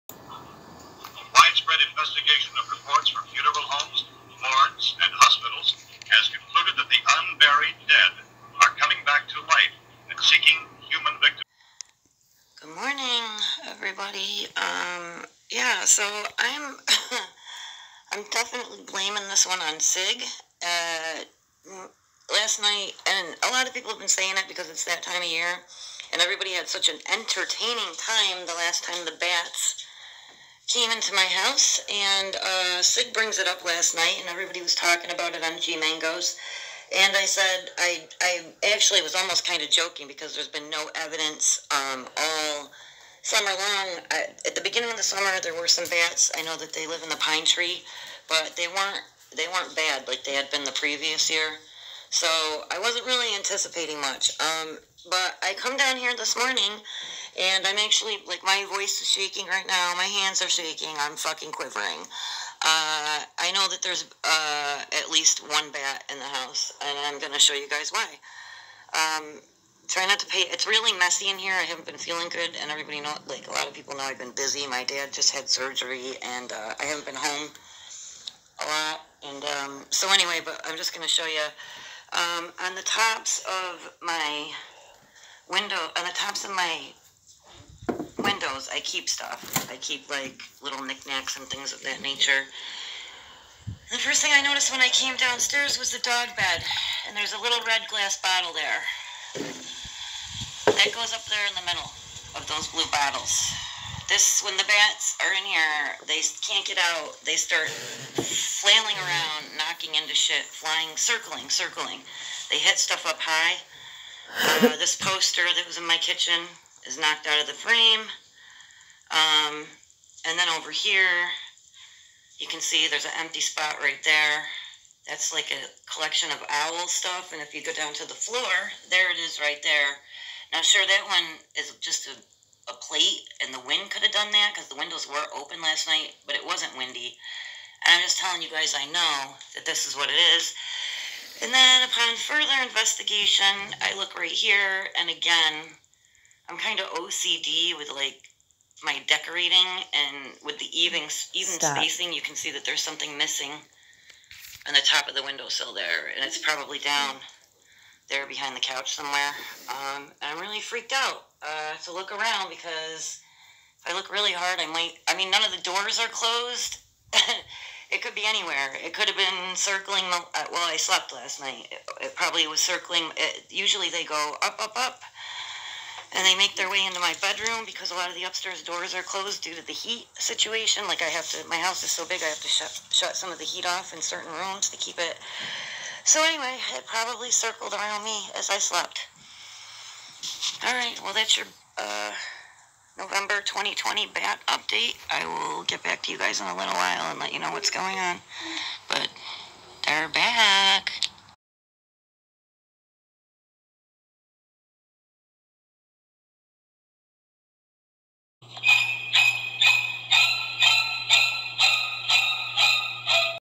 They're BAAAACK!!🦇🦇🦇🦇
In addition to my bat situation being discussed last night on G Mangos, there has also been a flurry of bat activity on my Twitter. I blame YOU ALL for summoning them as I have not spoken to Melton since our arguement last month. A lot of you know I just had a birthday and am getting pretty old at this point. This may just be the death of me. My heart cannot take it!!!